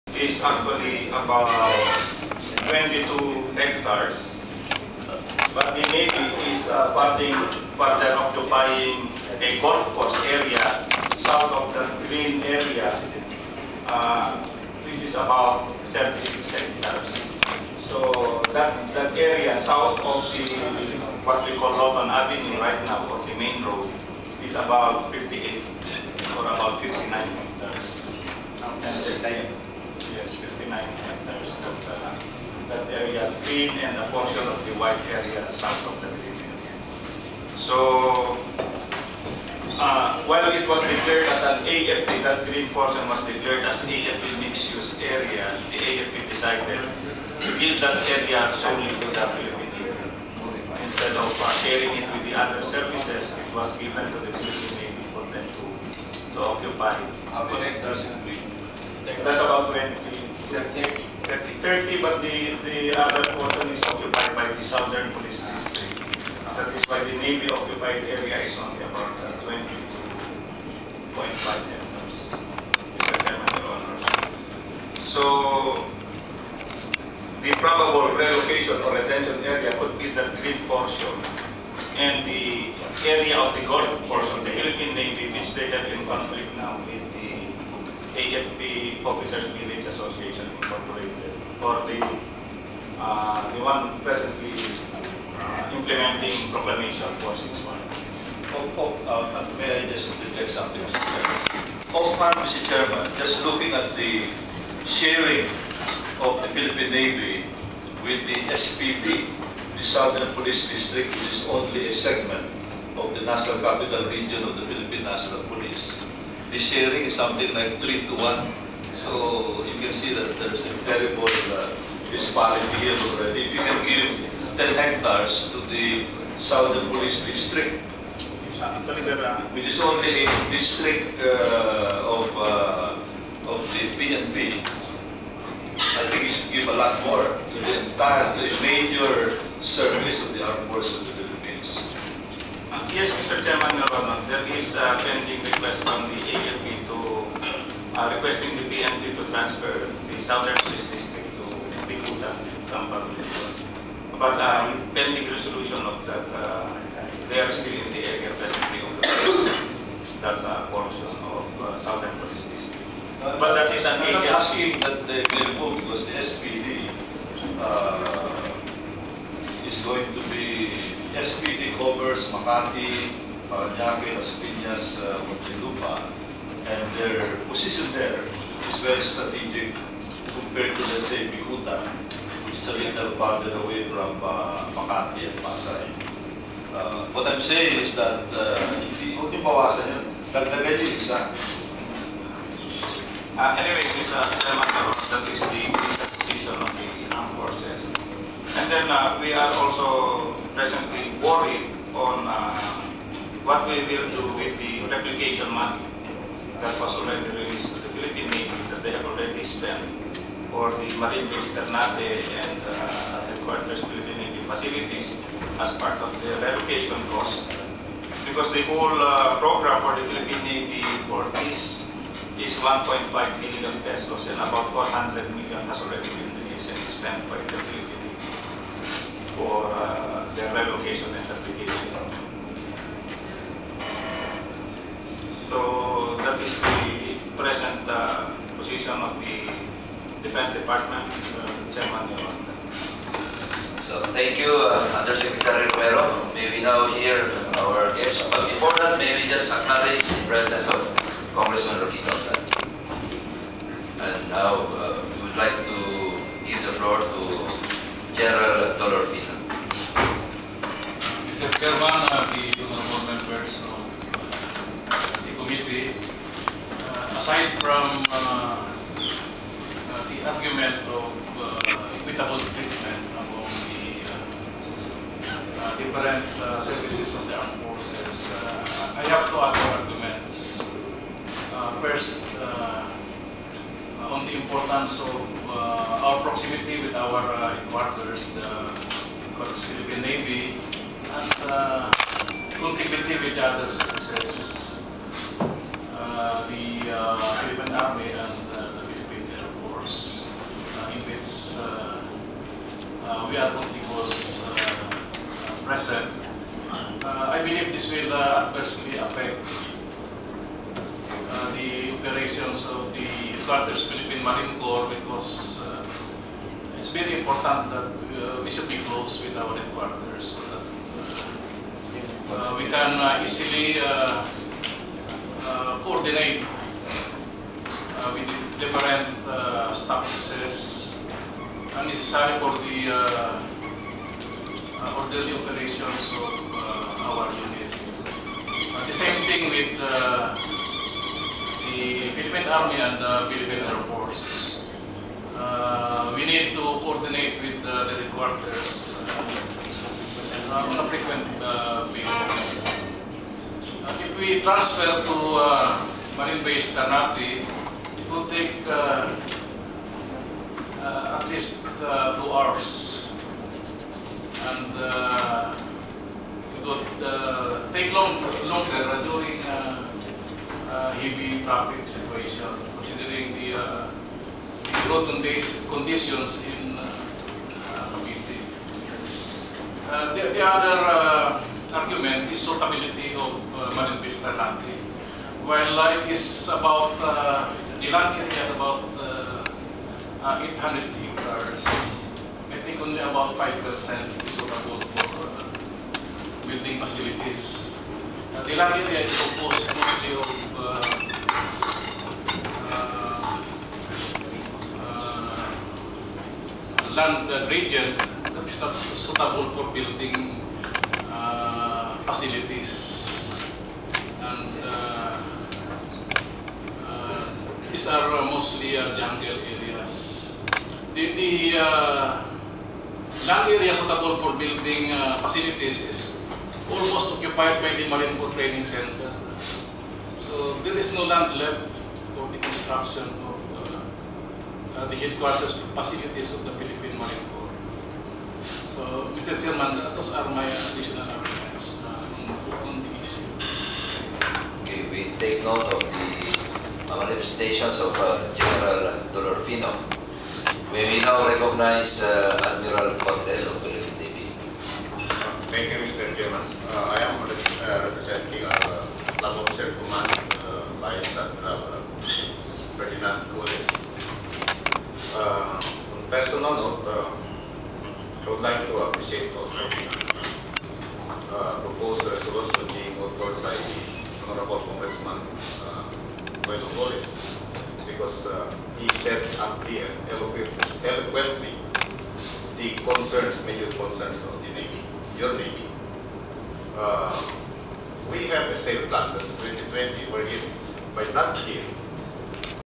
Philippine Navy, Marines land in Bonifacio, MGen. Dolorfino talks, 27 May 2009
Congressman Roilo Golez filed a House Resolution opposing the plan of BCDA to privatize most of the land occupied by the Philippine Navy and Philippine Marines in Fort Bonifacio. In this video, Major General Ben Dolorfino, Commandant, Philippine Marines, explains the position of the Philippine Marines.